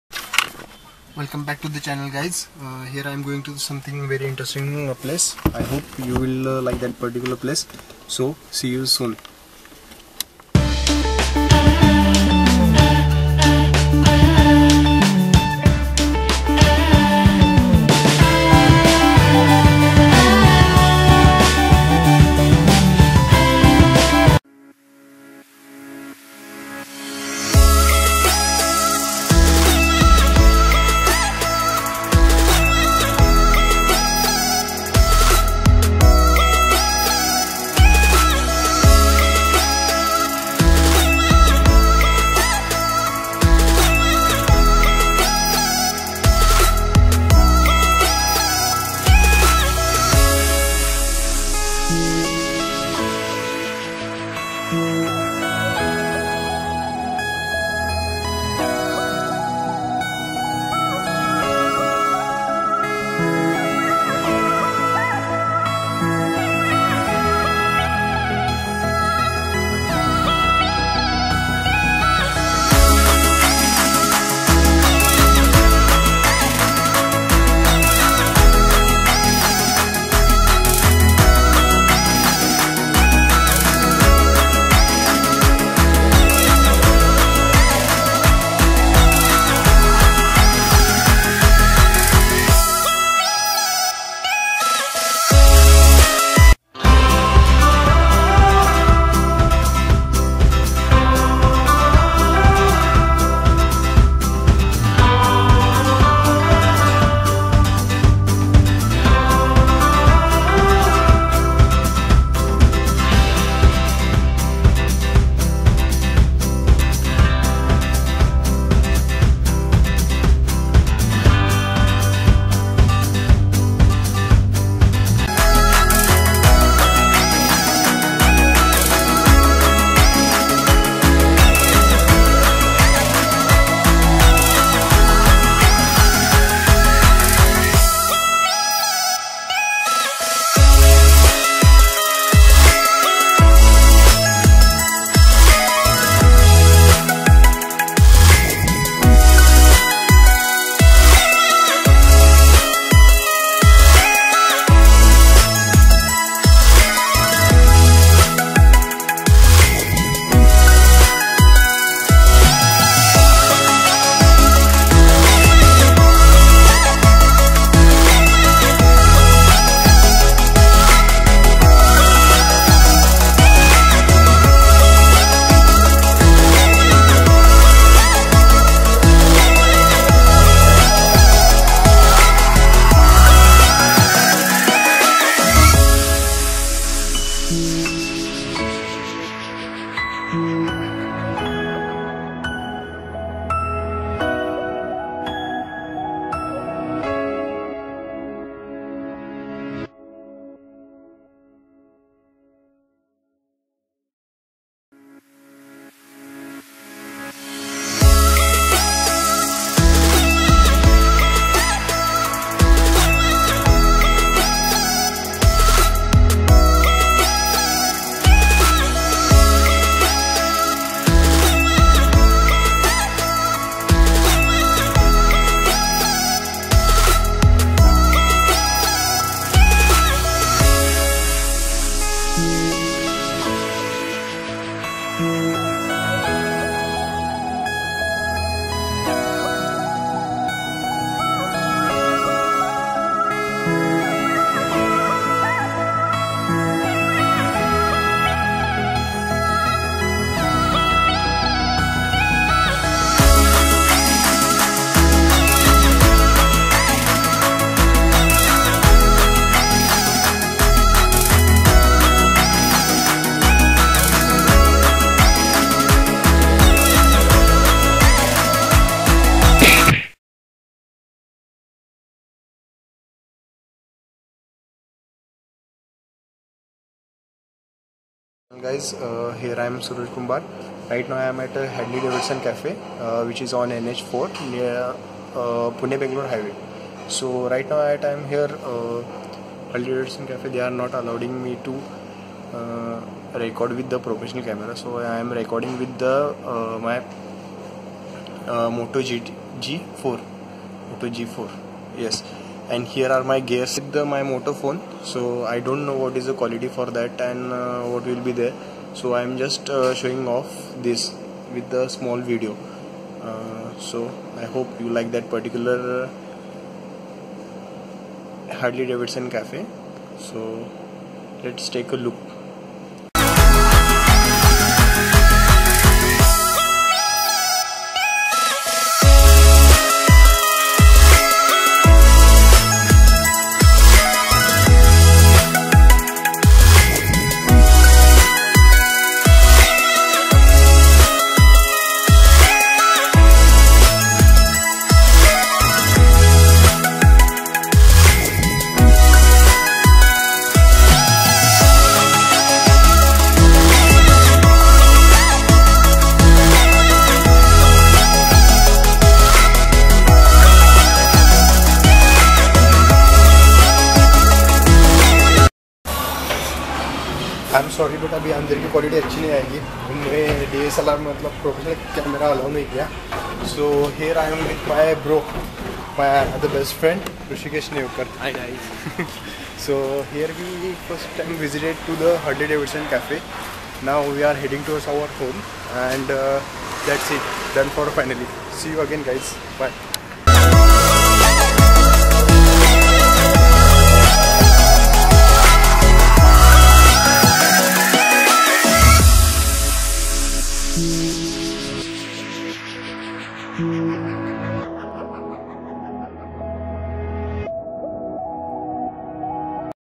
Harley Davidson Cafe Vlog 2
Finally Visited Most Loved "Harley Davidson Cafe".

If your travelling over NH4 From Pune to Banglore then must take here food break here.